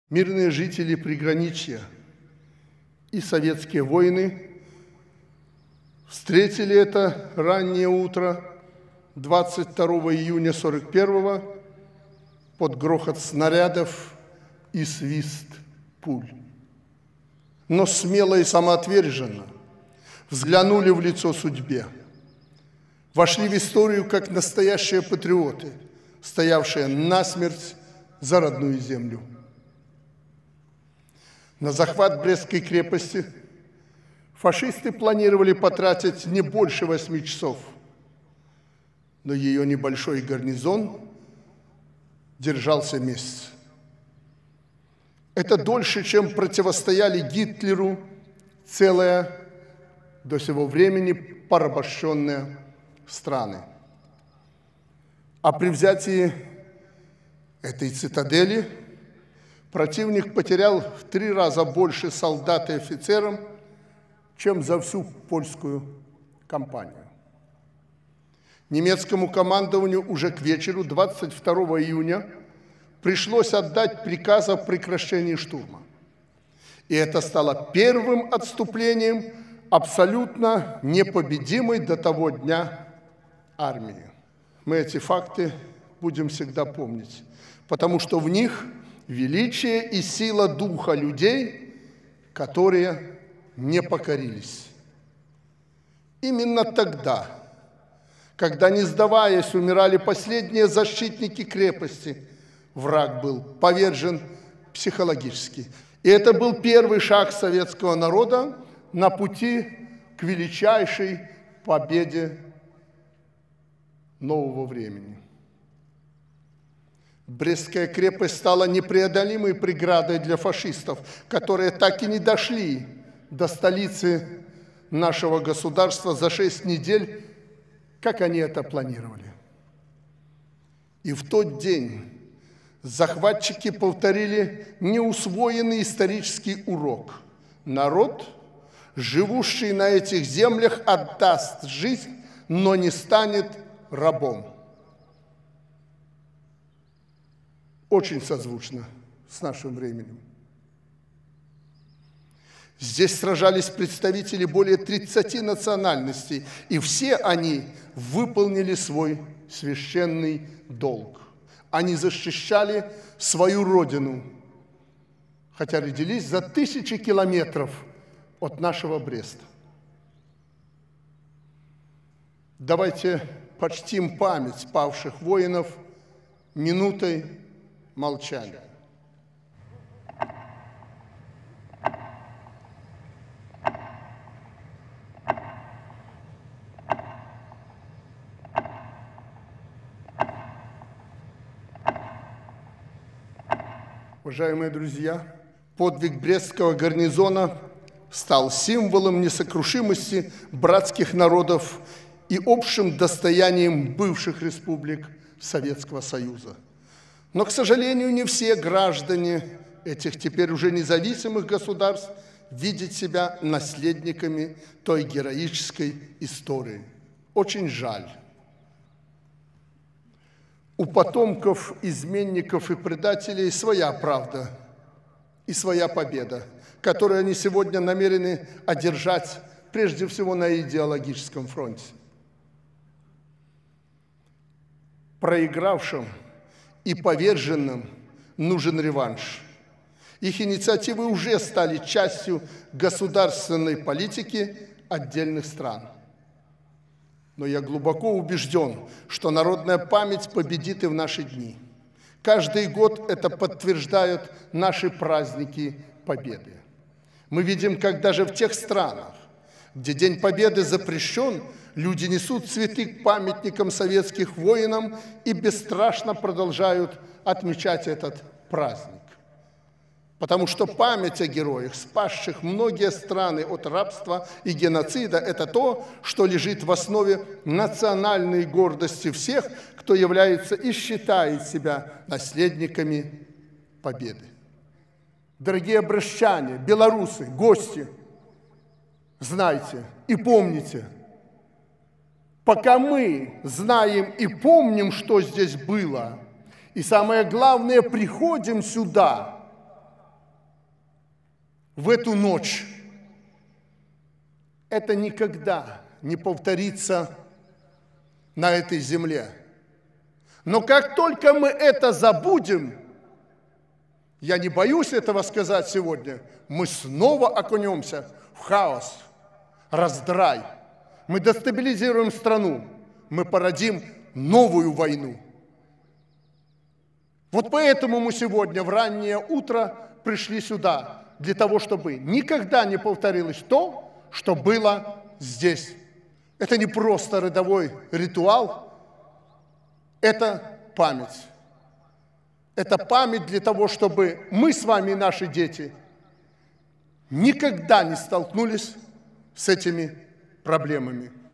Лукашенко: подвиг воинов Брестской крепости стал первым шагом на пути к Победе
Подвиг воинов Брестской крепости стал первым шагом советского народа на пути к Победе. Об этом Президент Беларуси Александр Лукашенко заявил в Брестской крепости на мероприятии, посвященном Дню всенародной памяти жертв Великой Отечественной войны.